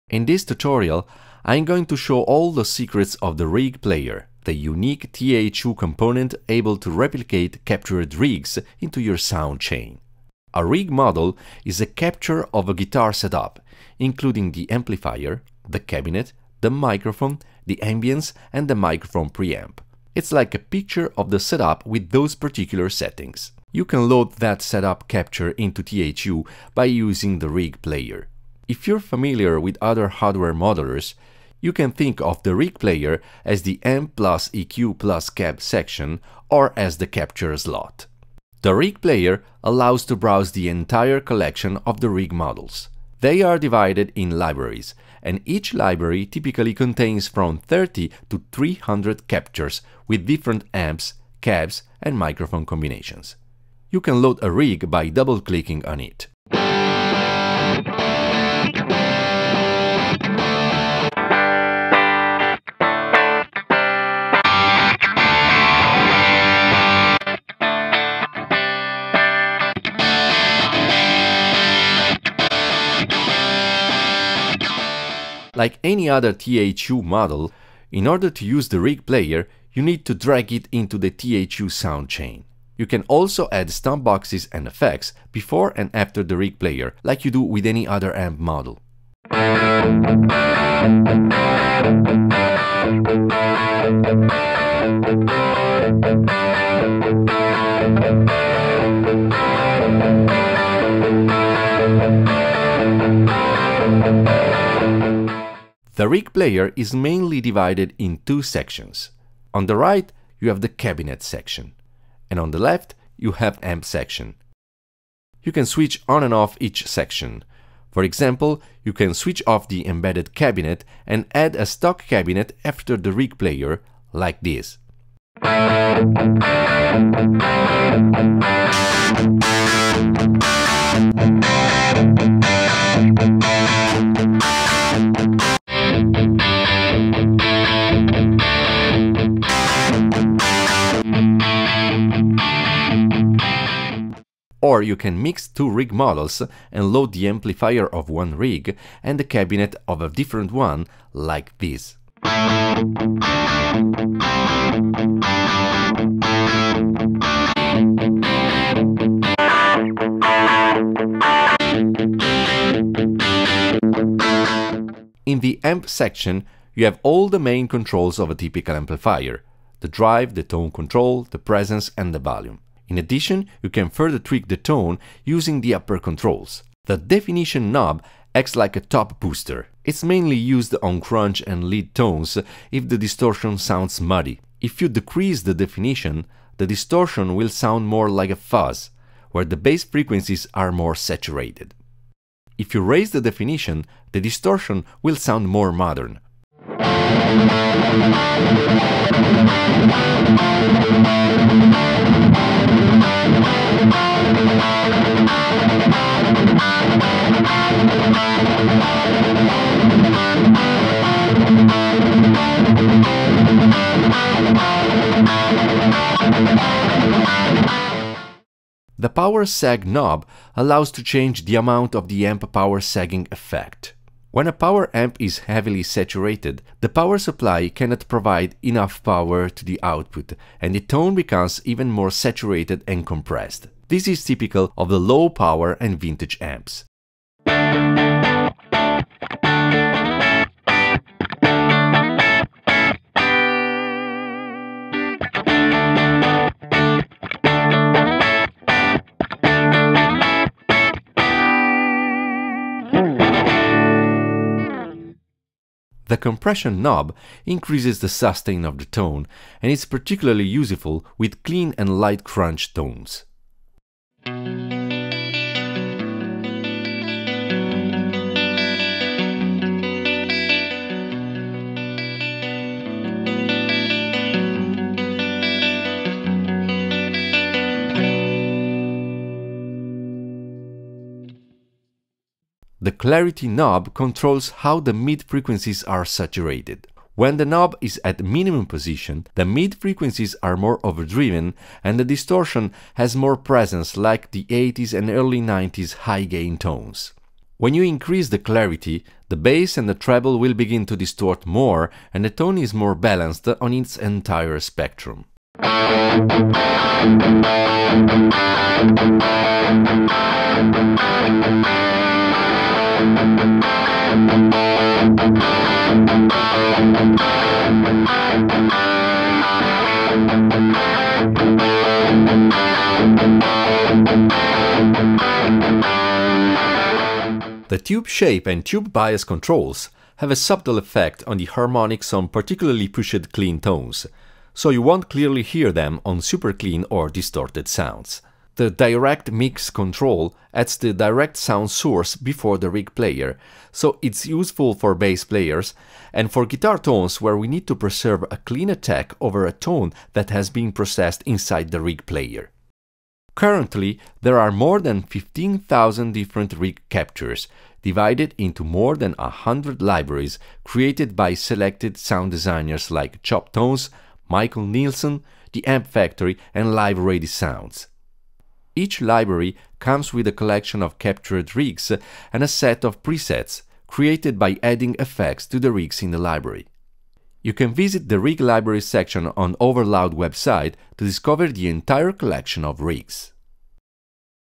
TH-U Rig Player - Discover the secrets of the TH-U Captured Rigs
TH-U is the first amp simulator software which features both amp modelling and amp capturing. The Rig Player is the component which allows to load a captured rig into the sound chain.
In this video you'll learn how to use the Rig Player, how to access to the largest library of captured setups ever created and how to tweak the captures.